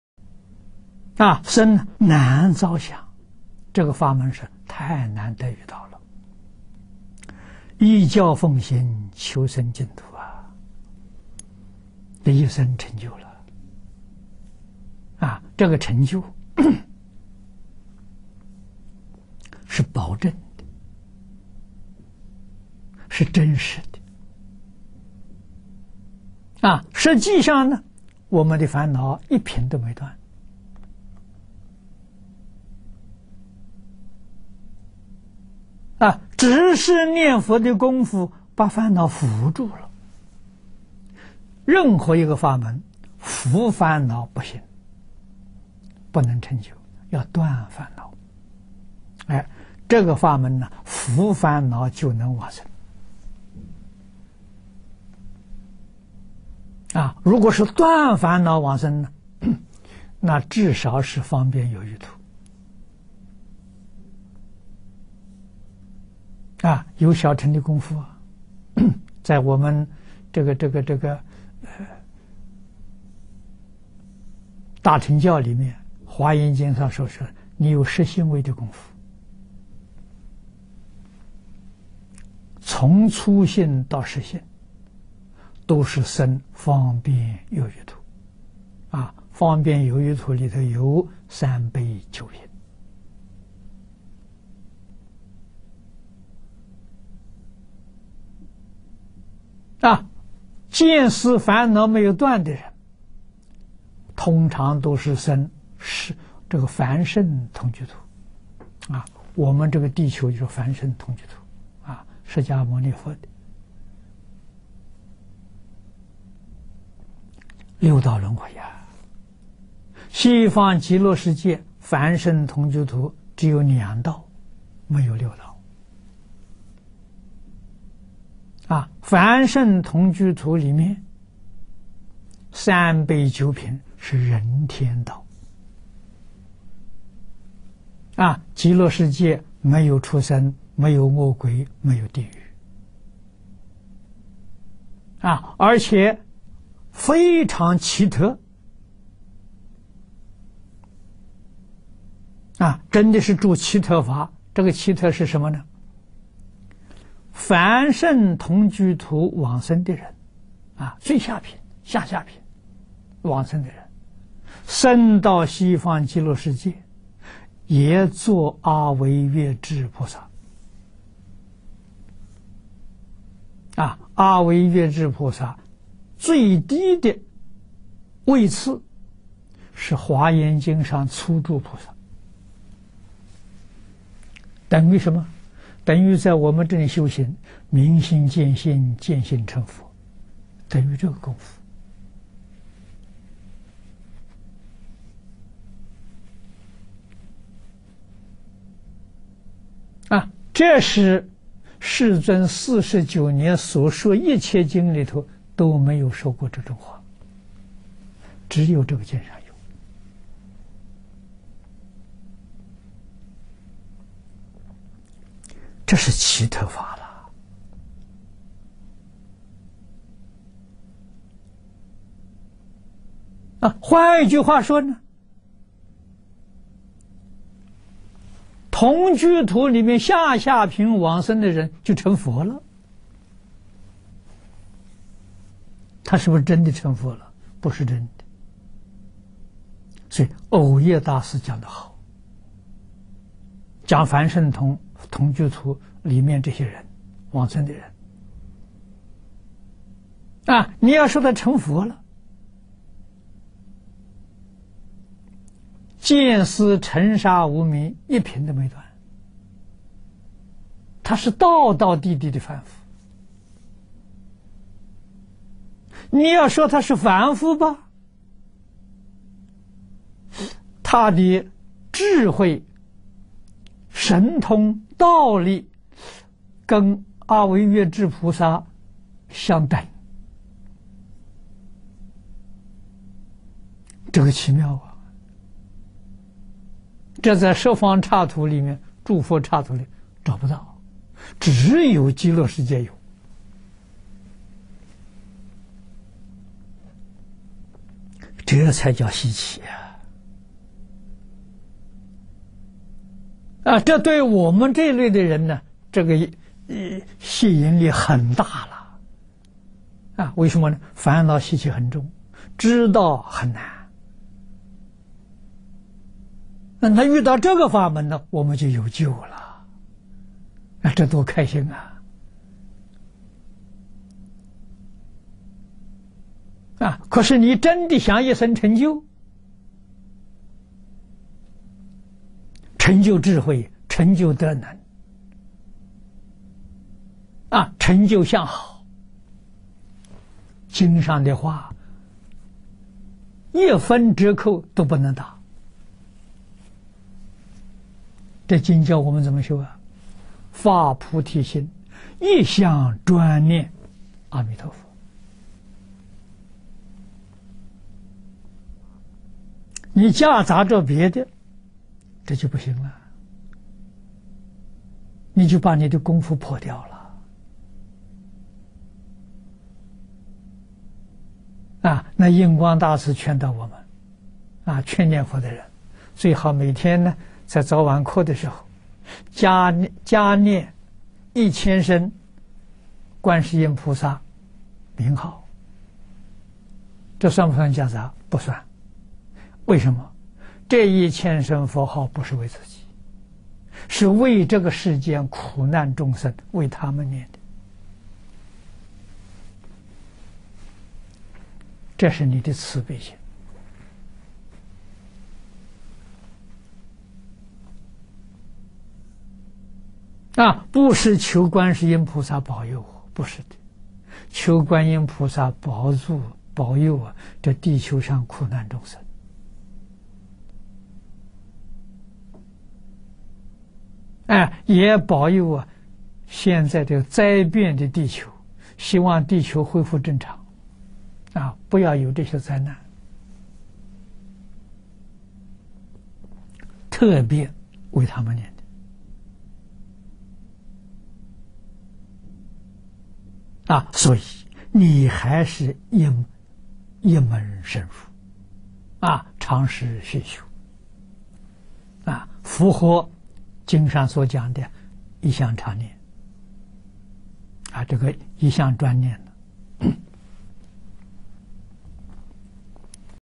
净空法师谈: 西方极乐世界的稀有奇特之处
文摘恭錄—淨土大經解演義（第一百三十八集）2010/9/9

「生難遭想」，這個法門是太難得遇到，「依教奉行，求生淨土」，這一生成就了。這個成就是保證的，是真實的。

實際上我們的煩惱一品都沒斷，只是念佛的功夫把煩惱伏住了。任何一個法門，伏煩惱不行，不能成就，要斷煩惱，這個法門伏煩惱就能往生。

如果是斷煩惱往生，那至少是方便有餘土，有小乘的功夫。在我們大乘教裡面，《華嚴經》上所說，你有十信位的功夫，從初信到十信都是生方便有餘土，方便有餘土裡頭有三輩九品。

見思煩惱沒有斷的人，通常都是生凡聖同居土。我們這個地球就是凡聖同居土，釋迦牟尼佛的，六道輪迴。西方極樂世界凡聖同居土只有兩道，沒有六道，凡聖同居土裡面三輩九品是人天道，極樂世界沒有畜生、沒有餓鬼、沒有地獄。
而且非常奇特，真的是「住奇特法」，這個奇特是什麼？凡聖同居土往生的人，最下品，下下品往生的人，生到西方極樂世界，也作阿惟越致菩薩。阿惟越致菩薩最低的位次是《華嚴經》上初住菩薩，等於什麼？等於在我們這裡修行，明心見性，見性成佛，等於這個功夫。

這是世尊四十九年所說一切經裡頭都沒有說過這種話，只有這個經上有，這是奇特法。換句話說，同居土裡面下下品往生的人就成佛了。他是不是真的成佛？不是真的。

所以蕅益大師講得好，講凡聖同居土裡面這些往生的人，你要說他成佛了，見思、塵沙、無明一品都沒斷，他是道道地地的凡夫；你要說他是凡夫，他的智慧、神通、道力跟阿惟越致菩薩相等，這個奇妙！這在十方諸佛剎土裡面找不到，只有極樂世界有，這才叫稀奇。

這對我們這類的人吸引力很大，為什麼？煩惱習氣很重，知道很難，遇到這個法門我們就有救了，這多開心！

可是你真的想一生成就，成就智慧、成就德能、成就相好，經上的話一分折扣都不能打。這經教我們怎麼修？發菩提心，一向專念阿彌陀佛。你夾雜著別的這就不行，你就把你的功夫破掉了。

印光大師勸導我們，勸念佛的人，最好每天在早晚課的時候加念一千聲觀世音名號。這算不算夾雜？不算。為什麼？這一千聲佛號不是為自己，是為這個世間苦難眾生，為他們念的，這是你的慈悲心。不是求觀世音菩薩保佑我，不是的，求觀音菩薩保佑這地球上苦難眾生，也保佑現在這個災變的地球，希望地球恢復正常，不要有這些災難，特別為他們念的。所以你還是一門深入，長時薰修，符合經上所講的一向專念。